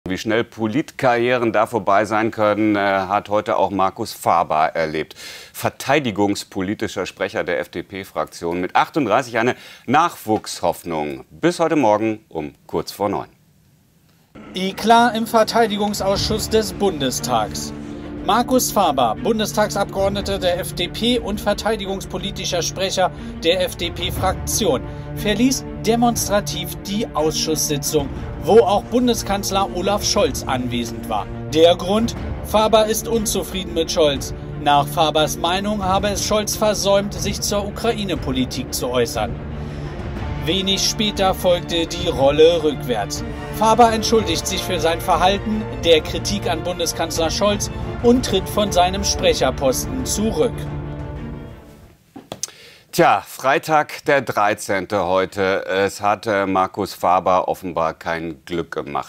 Ampel-Eklat bei Scholz Rede: FDP-Mann Marcus Faber schmeißt hin
Der verteidigungspolitische Sprecher der FDP-Fraktion, Marcus Faber (38), tritt nach dem Ampel-Eklat am Freitagvormittag von seinem Posten zurück – es gab Krach in der Fraktion! Bei Twitter schrieb Faber: „Die Kommentierung des heutigen Verteidigungsausschusses war unangemessen und wurde dem Ernst der Lage nicht gerecht. Dafür entschuldige ich mich und werde meiner Fraktion am Dienstag, in ihrer nächsten Sitzung, anbieten, von meinem Sprecherposten zurückzutreten.“

Bei der FDP „brannte die Hütte“ nach dem Eklat, wie ein FDP-Fraktionsmitglied anschließend zu BILD sagte. Parteichef Christian Lindner (43) habe getobt. Die Fraktions- und die Parteispitze habe extremen Gesprächsbedarf gehabt. Um 12 Uhr gab es eine Sondersitzung des Fraktionsvorstandes. Vermutlich war der interne Druck auch der Grund für die Kehrtwende; nach BILD-Informationen war dieser in den vergangenen Stunde immer weiter gestiegen. Dann reichte es ihm offenbar: Faber schmiss hin.

Doch was war eigentlich passiert? Die FDP-Arbeitsgruppe hatte die Sondersitzung des Verteidigungsausschusses am Freitagvormittag aus Protest gegen die Antworten von Bundeskanzler Olaf Scholz (63, SPD) geschlossen verlassen – und das, als der Kanzler noch sprach. Darüber hatte BILD zunächst exklusiv berichtet.

Nach der Sitzung sagte Faber in die Mikros von ARD-Journalisten: „Leider wurden sehr viele Fragen (…) nicht beantwortet“. Und dann: „Deswegen haben wir als Freie Demokraten um kurz nach 9 Uhr entschieden, dass wir die Sitzung jetzt verlassen.“

Danach ruderte die Partei plötzlich zurück – auch FDP-Politiker Faber. „Es tut mir sehr leid, dass ein anderer Eindruck entstanden ist, den ich hiermit entschieden zurückweise“, schrieb er in einer offiziellen Mitteilung und erklärte: „Wegen Anschlussterminen mussten die Mitglieder meiner Fraktion nach und nach die Ausschusssitzung verlassen.“